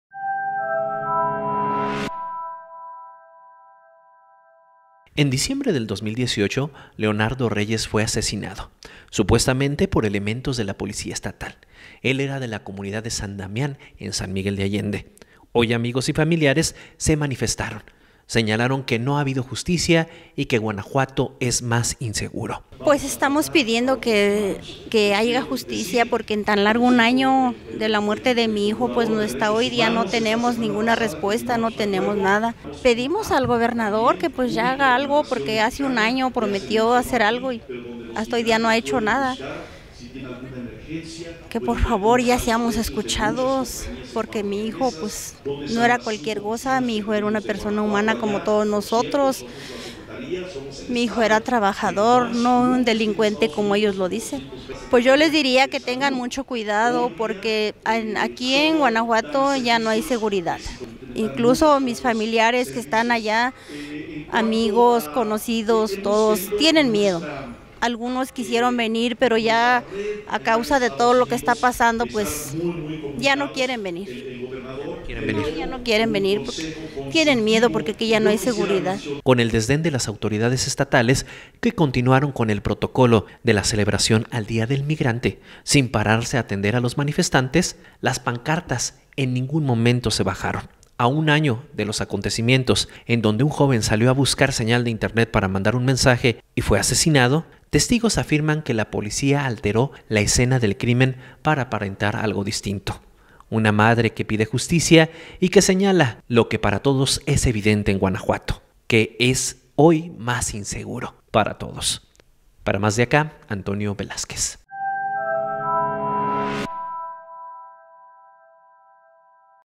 A un año, familia de Leo Reyes sigue pidiendo justicia
Dentro de la celebración del Día del Migrante se manifestaron, la indiferencia de gobierno estatal se hizo presente, la madre de Leo señala que sus familiares en E.E.U.U. ya tienen miedo de venir a Guanajuato